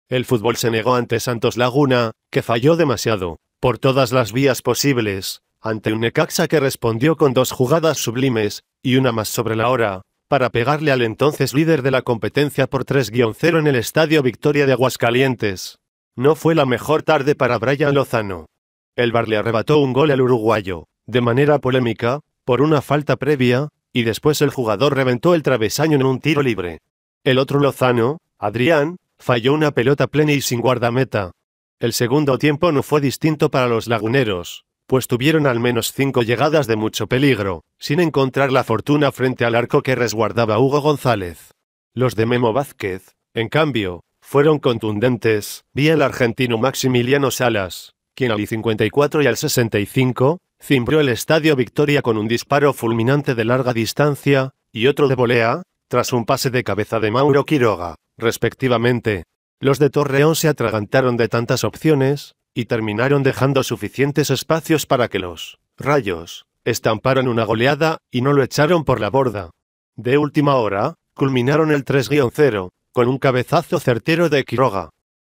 Necaxa golea 3-0 a Santos y le quita el invicto en la Jornada 5 Apertura 2019
El futbol se negó ante Santos Laguna, que falló demasiado, por todas las vías posibles, ante un Necaxa que respondió con dos jugadas sublimes, y una más sobre la hora, para pegarle al entonces líder de la competencia por 3-0 en el Estadio Victoria de Aguascalientes.

No fue la mejor tarde para Brian Lozano. El VAR le arrebató un gol al uruguayo, de manera polémica, por una falta previa, y después el jugador reventó el travesaño en un tiro libre. El otro Lozano, Adrián, falló una pelota plena y sin guardameta. El segundo tiempo no fue distinto para los laguneros, pues tuvieron al menos cinco llegadas de mucho peligro, sin encontrar la fortuna frente al arco que resguardaba Hugo González.

Los de Memo Vázquez, en cambio, fueron contundentes, vía el argentino Maximiliano Salas, quien al y 54 y al 65, cimbró el Estadio Victoria con un disparo fulminante de larga distancia, y otro de volea, tras un pase de cabeza de Mauro Quiroga, respectivamente.

Los de Torreón se atragantaron de tantas opciones, y terminaron dejando suficientes espacios para que los ‘Rayos’ estamparan una goleada, y no lo echaron por la borda. De última hora, culminaron el 3-0, con un cabezazo certero de Quiroga.